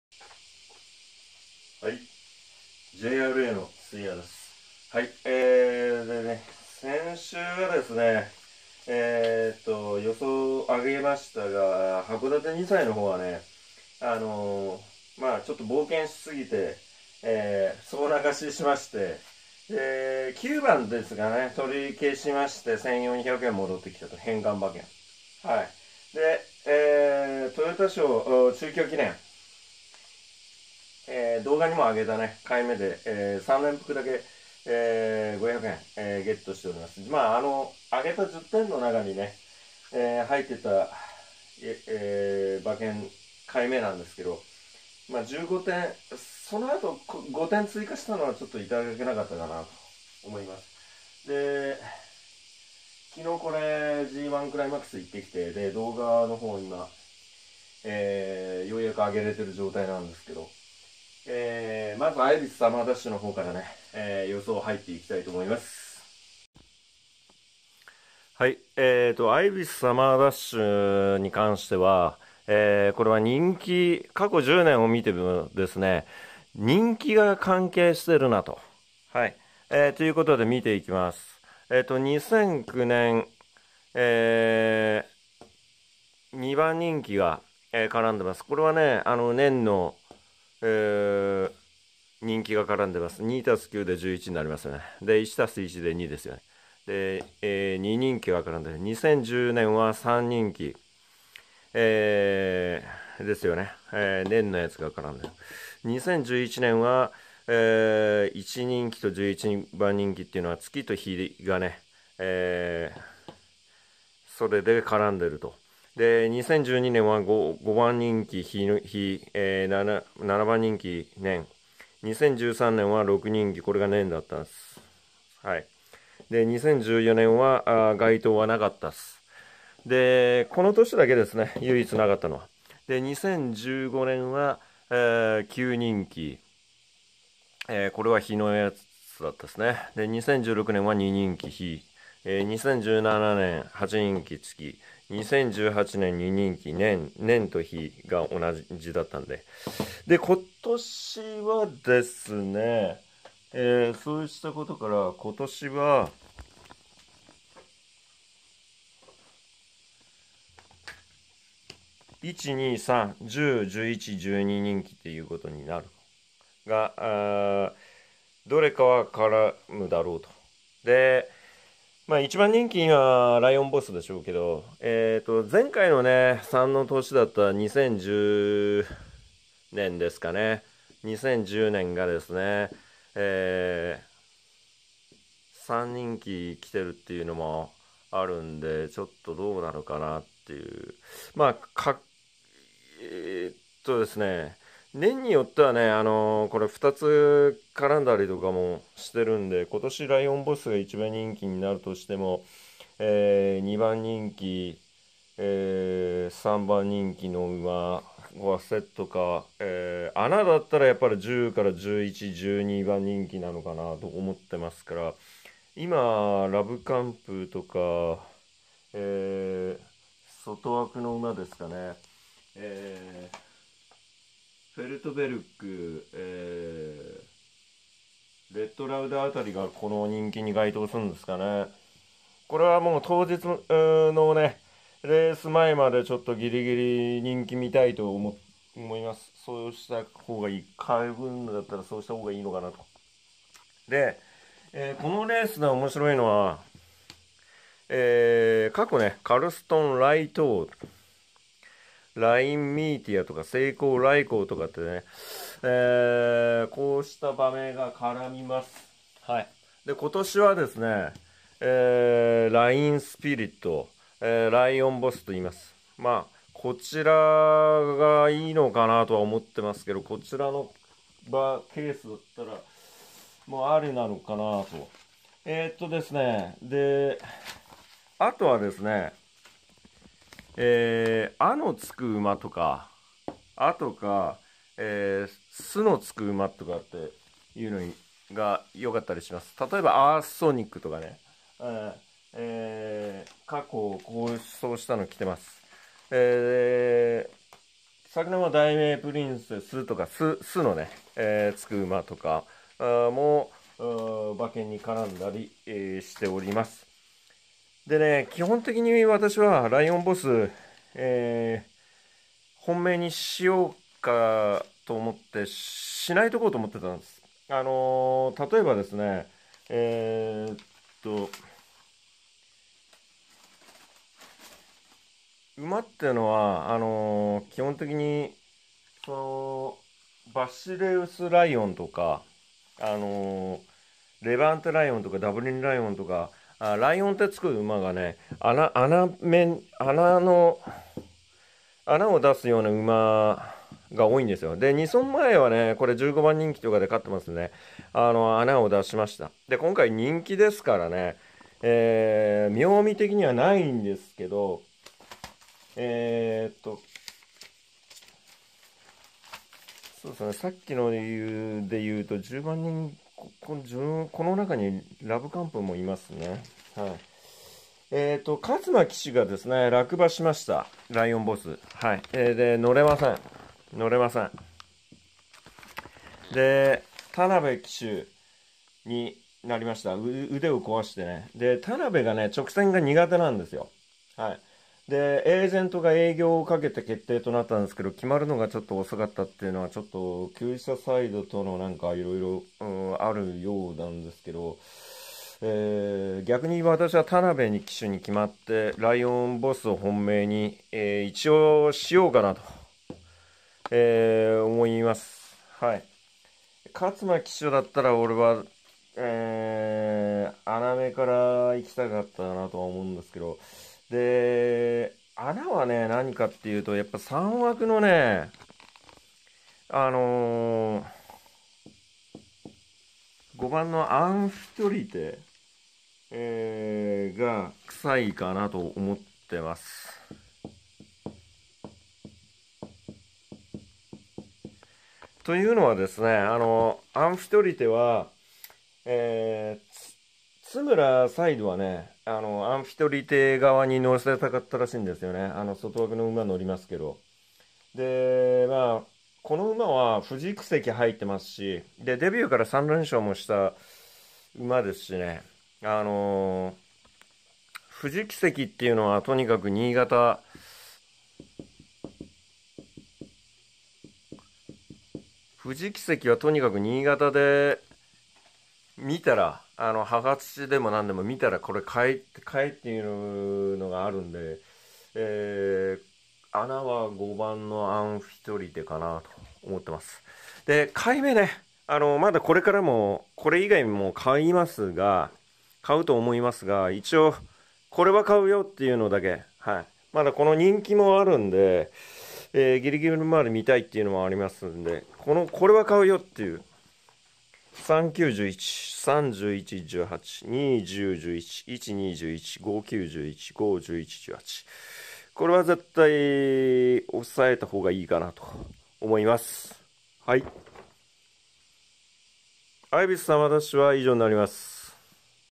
【第19回アイビスSD(予想)】“特注文字”と“人気”に注意を！！
過去の好走馬名をみれば、傾向が出ている。

今年なら…該当馬は！？

それと、このレースはなんといっても人気に注意！！

さ～て、3週連続的中なるか！！？